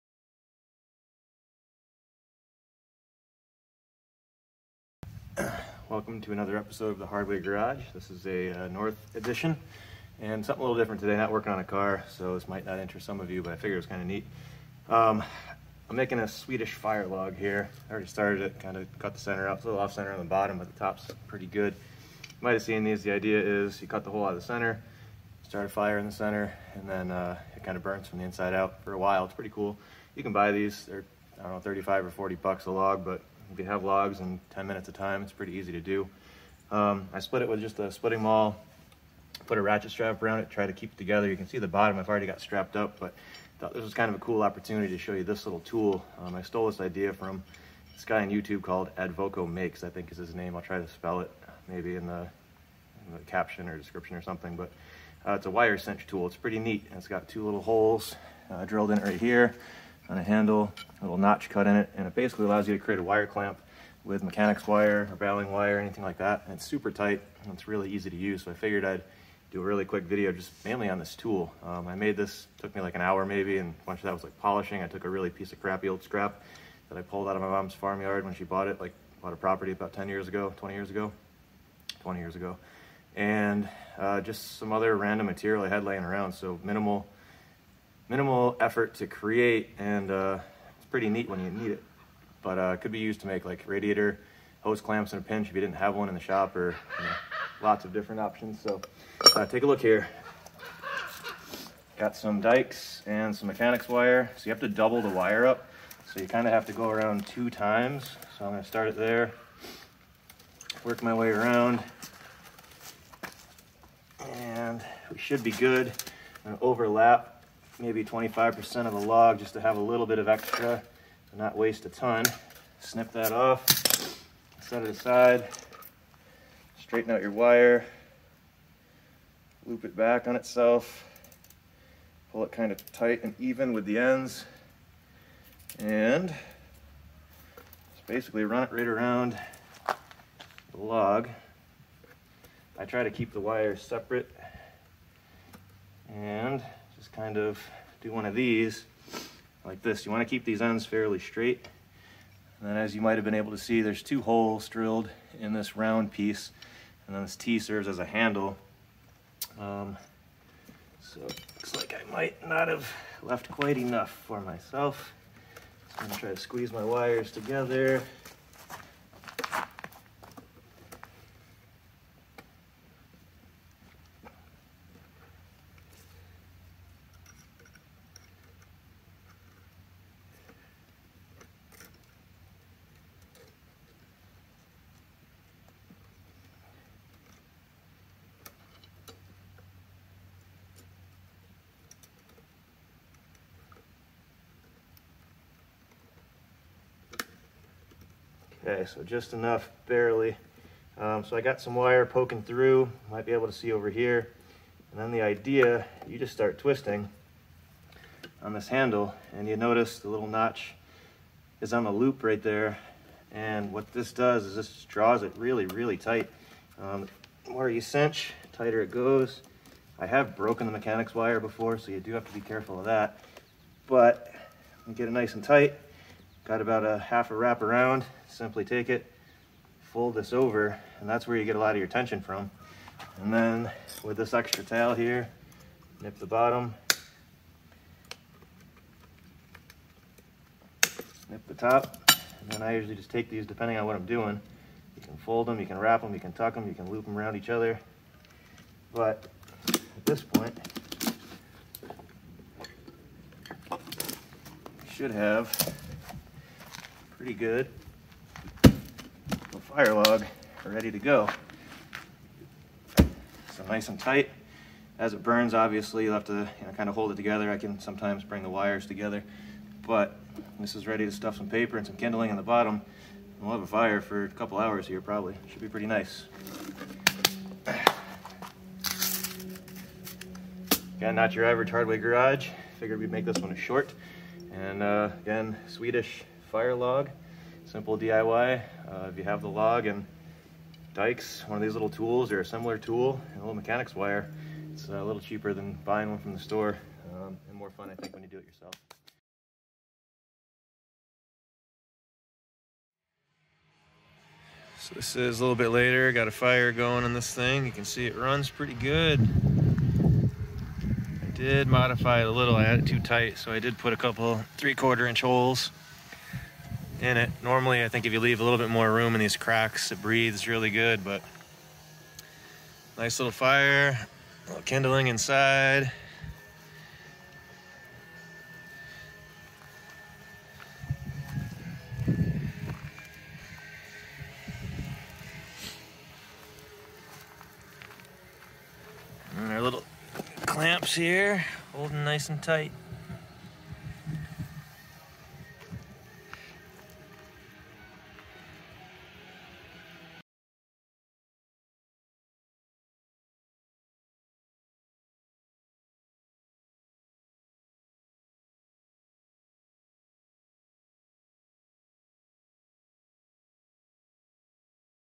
Swedish Fire Log and Mechanics Wire Cinch | Shorts
This time on The Hardway Garage we are straying from automotive for an episode. I will demo a simple and cheap Swedish fire log and how to use a homemade wire cinch tool